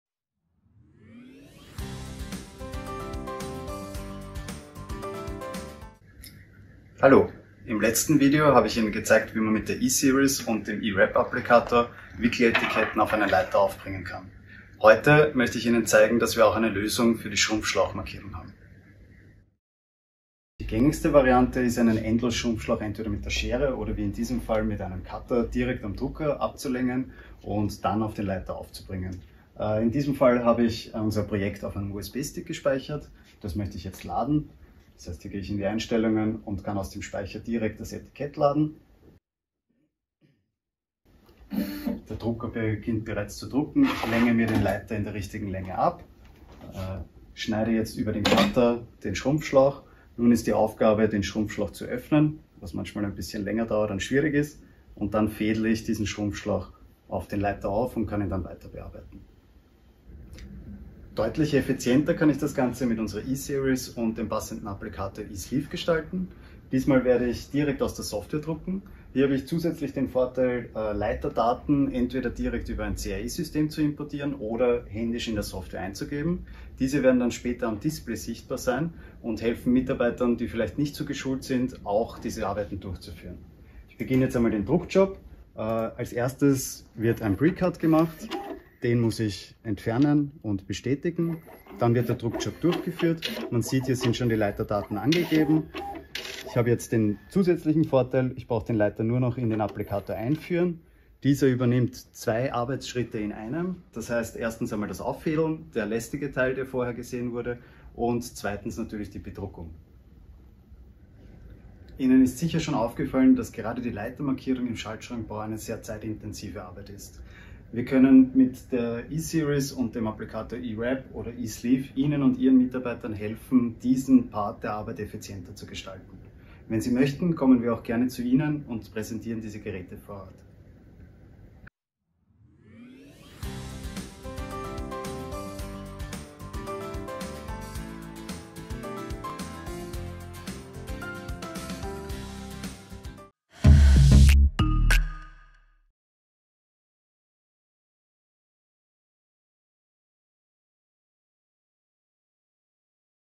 Schnelle Schrumpfschlauchbedruckung und -aufbringung mit dem THERMOMARK ESERIES E.SLEEVE
►Mit dem Thermodrucker ESERIES E.600D sparen Sie Zeit und vermeiden Fehler bei der Schrumpfschlauchmarkierung und -aufbringung. Mit dem E.Sleeve Adapter bringen auch ungelernte oder wenig erfahrene Mitarbeiter den Schrumpfschlauch schnell an. Der Schrumpfschlauch wird nach dem Drucken vom Thermodrucker direkt auf den Leiter appliziert. Wie einfach das geht, sehen Sie in unserem Video.